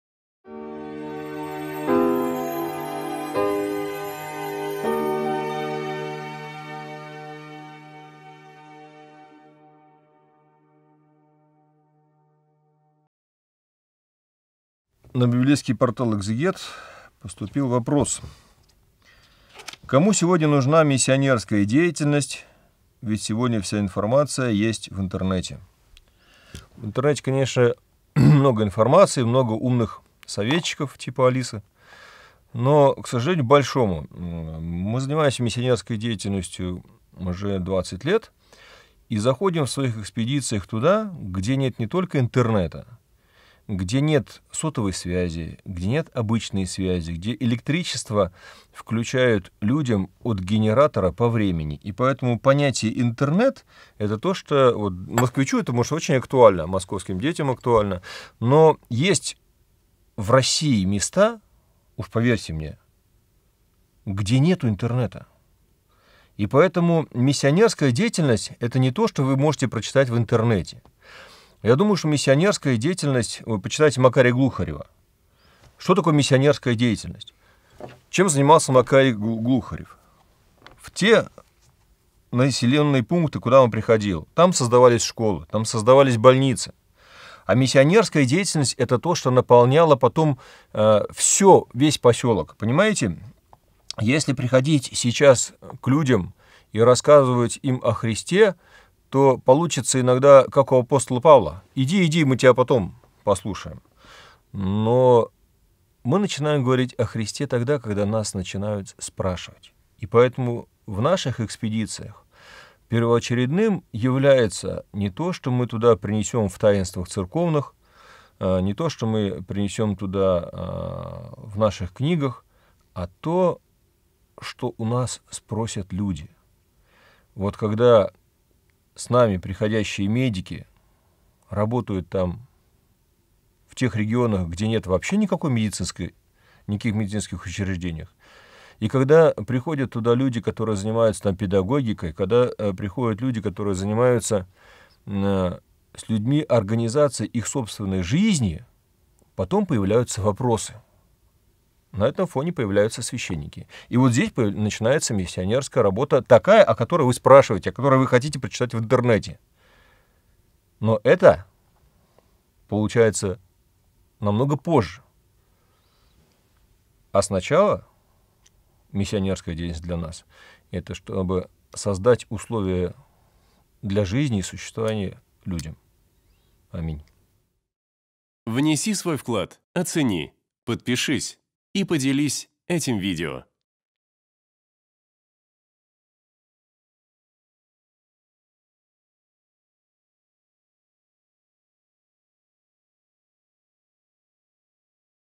Зачем миссионерская деятельность, если вся информация есть в интернете? о.Александр Тылькевич
В сегодняшнем видео вместе с протоиереем Александром Тылькевичем отвечаем на вопрос: "Зачем миссионерская деятельность, если вся информация есть в интернете?" Касаемся следующих тем:

🔸 Отдаленные уголки России.
🔸 Архимандрит Макарий.
🔸 Условие для проповеди.